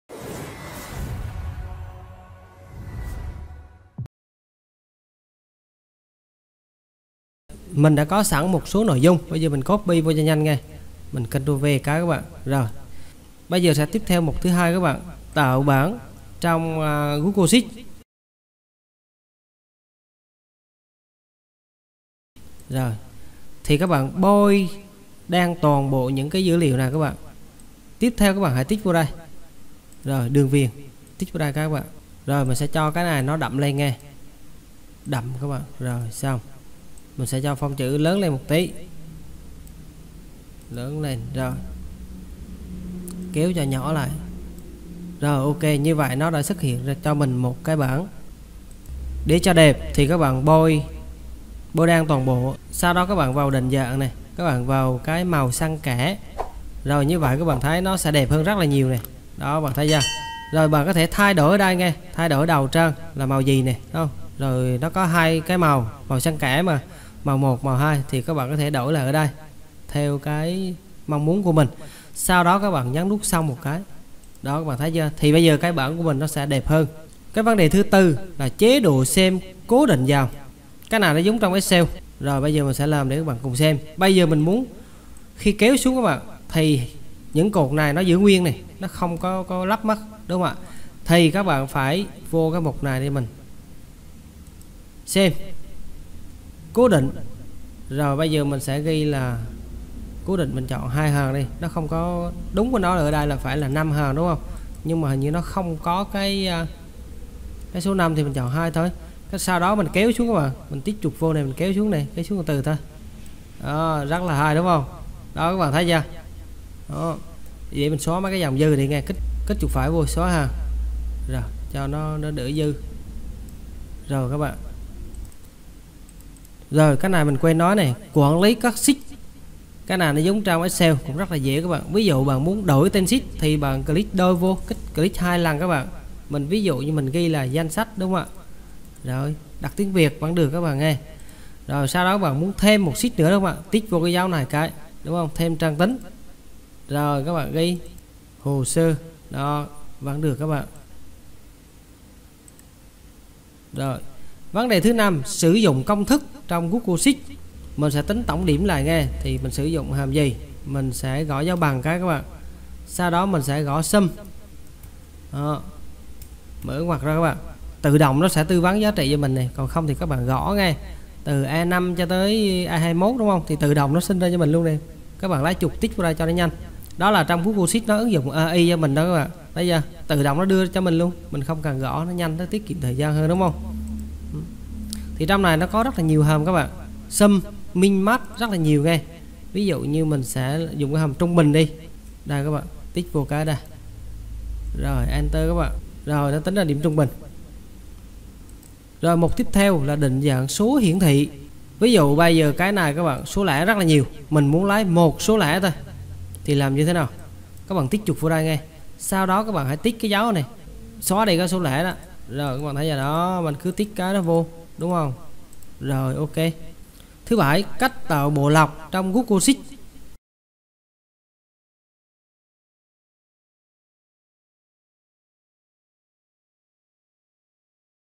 Tạo table và định dạng màu | Học google sheet | Phần 2 | NT Software
Tạo table và định dạng màu | Học google sheet | Phần 2
00:10 copy dữ liệu từ file excel sang google sheet
00:17 Tạo bảng trong google sheet
00:48 Dịnh dạng màu bảng (table)
01:19 Chế độ xem cố định dòng trong google sheet
02:08 Quản lý sheet trong google sheet
02:36 Sử dụng công thức trong  google sheet

► Nhận thêm nhiều video về thủ thuật phần mềm nhanh nhất:
► Powered by NT SOFTWARE ©. Vui lòng không đăng tải lại dưới bất kì hình thức nào.